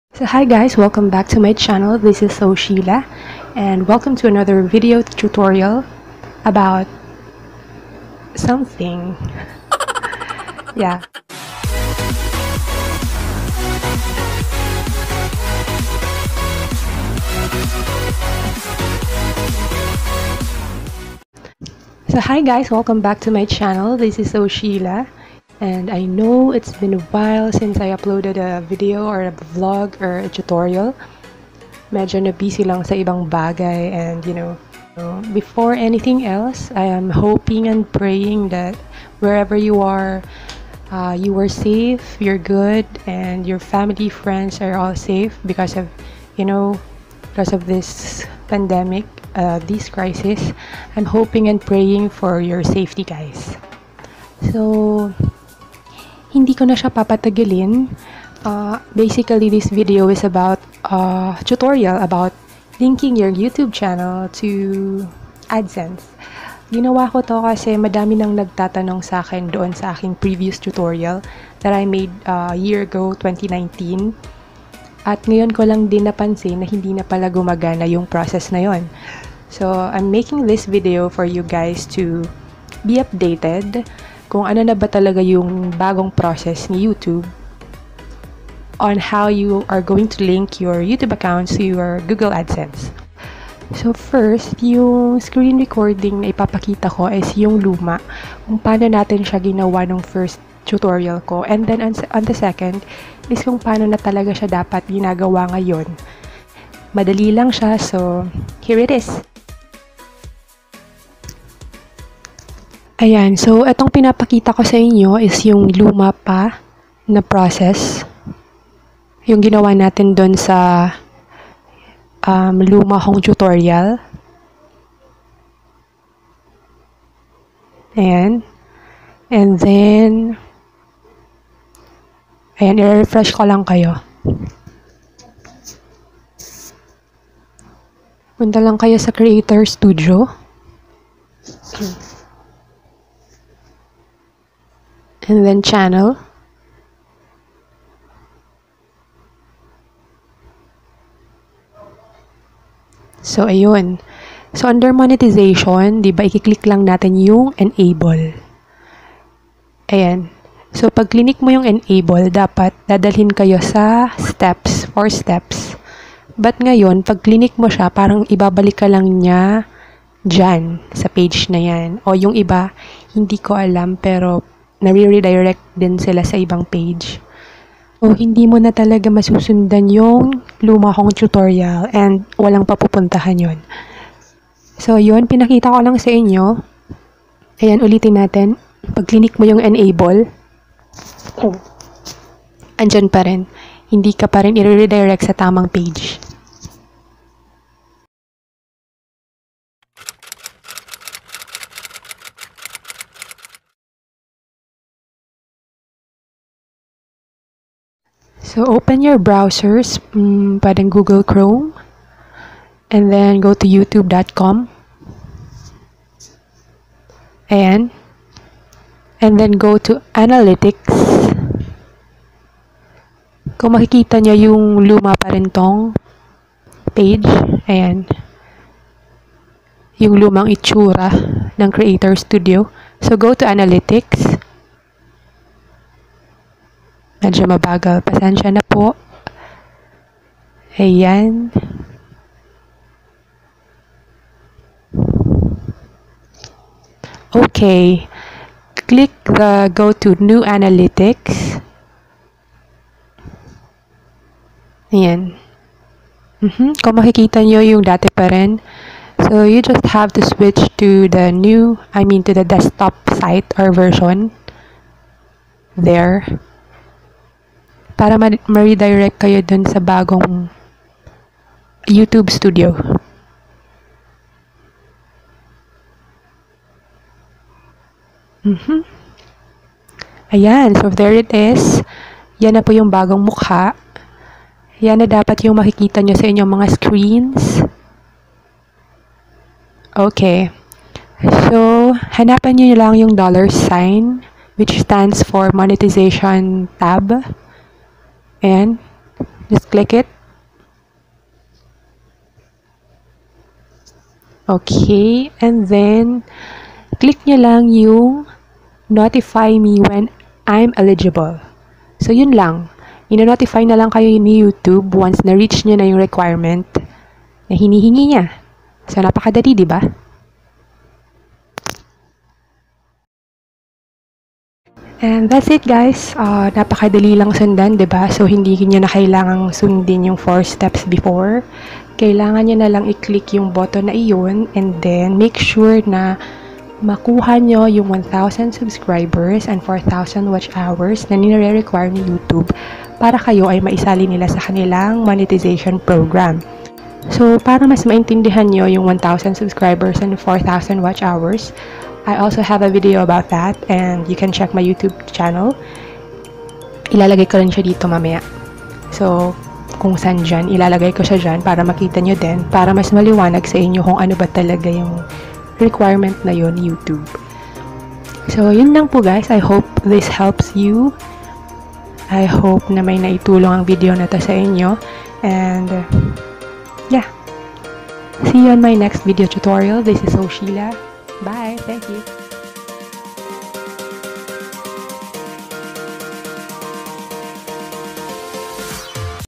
How to link Google Adsense to Youtube channel using mobile phone 2020 | STEP BY STEP GUIDE
UPDATED.
EASY GUIDE/TUTORIAL on how to link GOOGLE ADSENSE to your YOUTUBE ACCOUNTS. 2020

Click the link below for the explanation of the youtube requirement: 1,000 SUBSCRIBERS and 4,000 watch hours